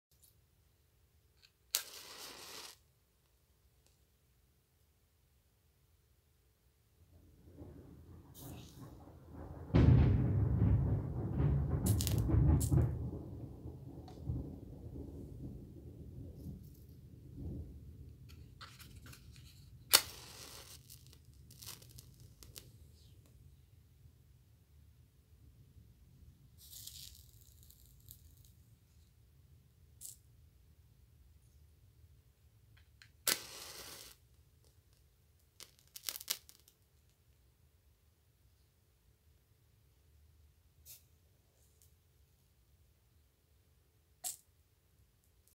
Thunder Fright of filming the burning matches video.
The thunder was so loud while I was filming this video that I got a fright!🤣asmr